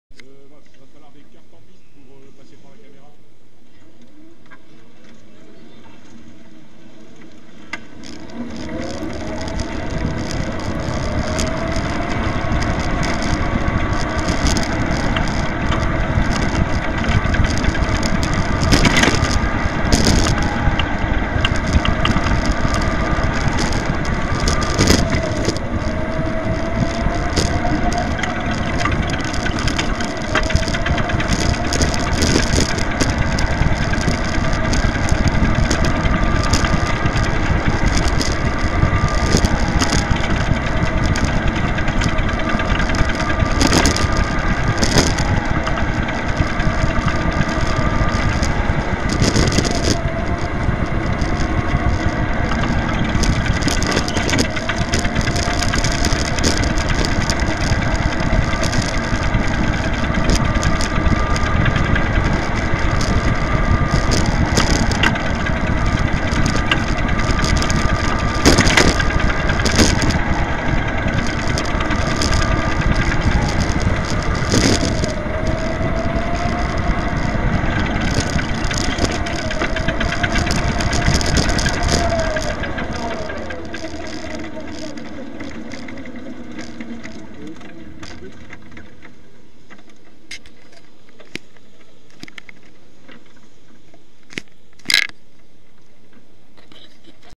TELETHON 2013 Vierzon - Tour de piste en kart électrique
Tours de piste en kart électrique à l'occasion des 24 heures en karts électriques, du vendredi 7 décembre à 18 heures au samedi 8 à la même heure, au Pavillon du Parc des Exposition de Vierzon, au profit du Téléthon 2013.